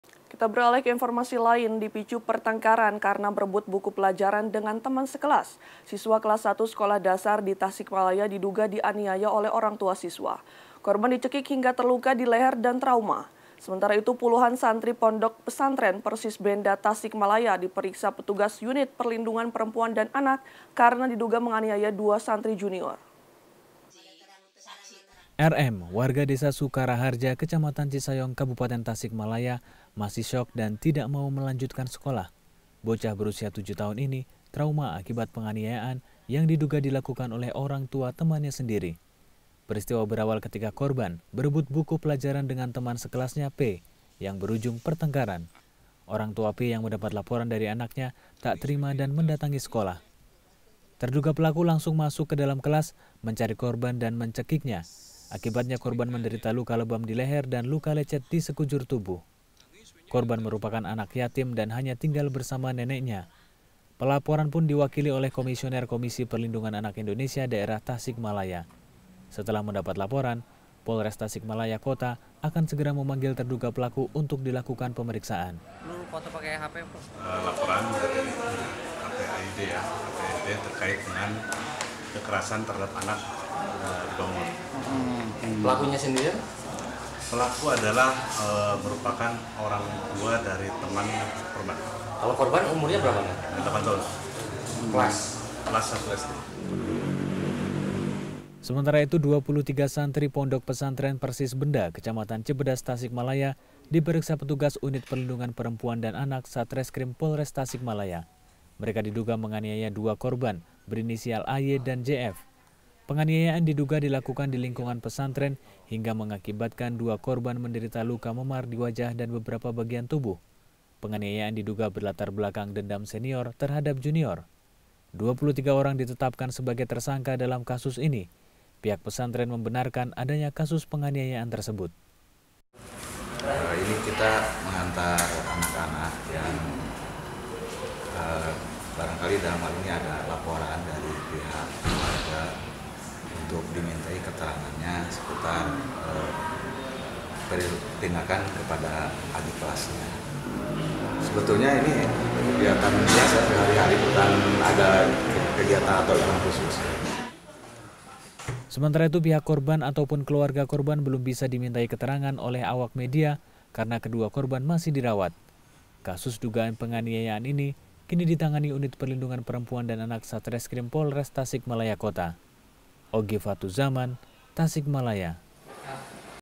Seorang Siswa SD Dianiaya Orang Tua Temannya, Penganiayaan Sesama Santri
Dipicu pertengkaran karena rebutan buku pelajaran dengan teman sekelasnya, siswa kelas satu sekolah dasar di Tasikmalaya, diduga dianiaya oleh orangtua siswa. Korban dicekik hingga terluka di leher dan trauma.

Sementara itu puluhan santri Pondok Pesantren Persis Benda, Tasikmalaya, diperiksa petugas Unit Perlindungan Perempuan dan Anak, karena diduga menganiaya dua santri junior.

Follow IG kami di:
transmediajabar